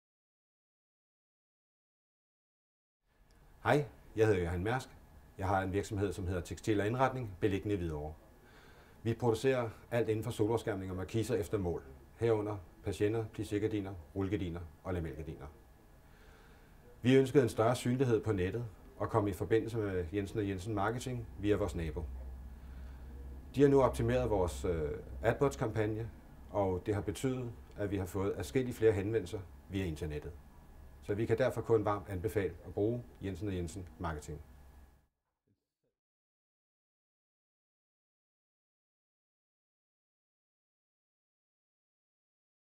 Videoreference Jensen & Jensen Marketing
Videoreference med Johan Mærsk fra Textil & Indretning, Hvidovre, der anbefaler andre erhvervsdrivende at bruge Jensen & Jensen Marketing til, at lave deres AdWordskampagner.